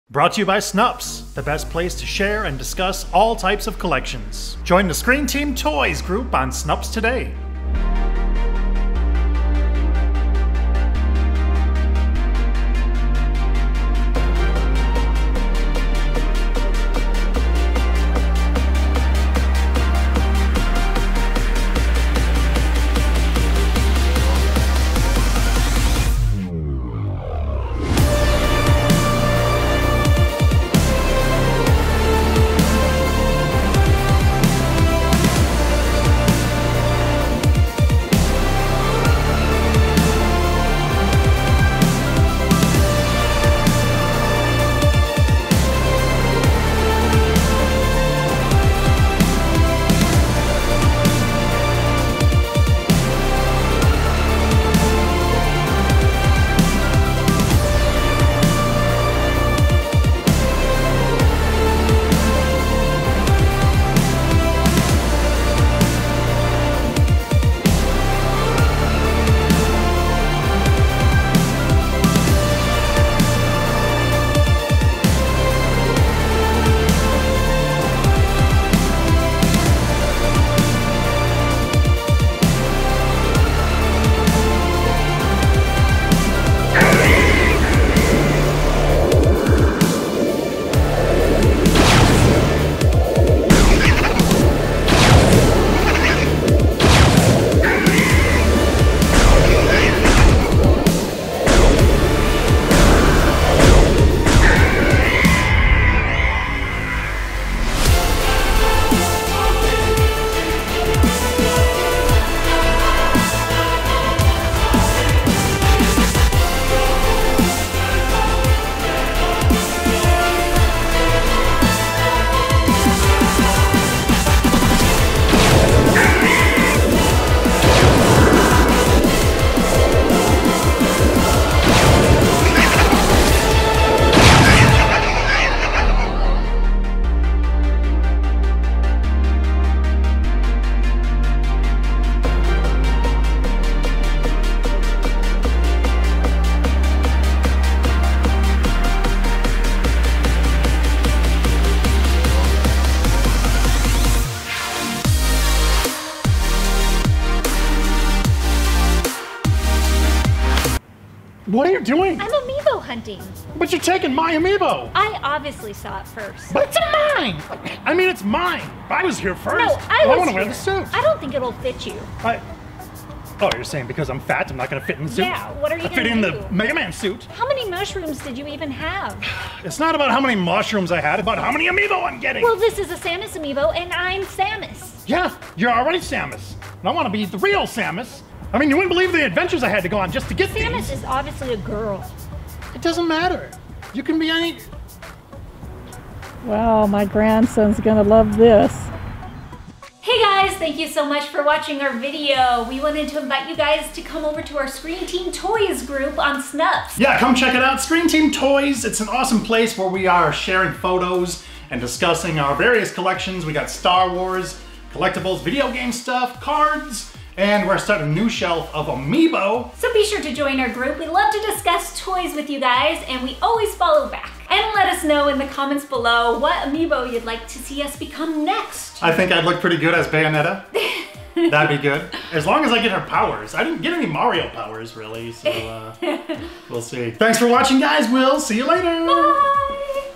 Samus Aran (Metroid): The Story You Never Knew Amiibo Hunting in latex Cosplay | Screen Team
Join the SCREEN TEAM TOYS GROUP on Snupps today

Angie, as Zero Suit Samus from Metroid goes on an Amiibo hunt
Immerse yourself in a story of Metroid legend, the tale of Samus Aran. Understand this stoic Nintendo bounty hunter's story, where she came from and how she became the complete bad ass we know today. Uncover a true understanding of Samus, the story you never knew.

Write us!